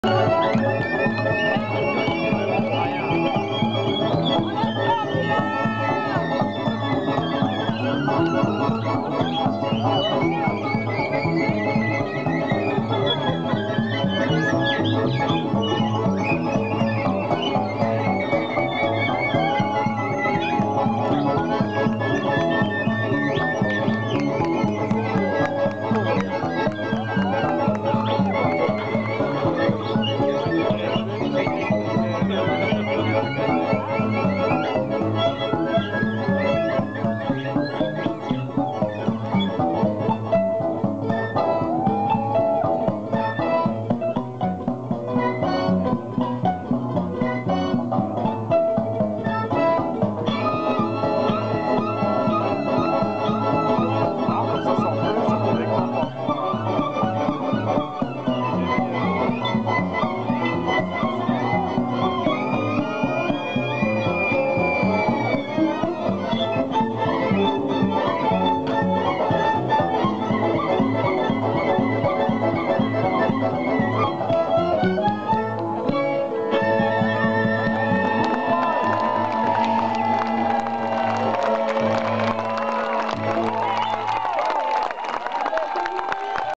Taraf De Haïdouks in Pont Du Gard
live de Taraf De Haïdouks au Pont Du Gard au Festival Radio France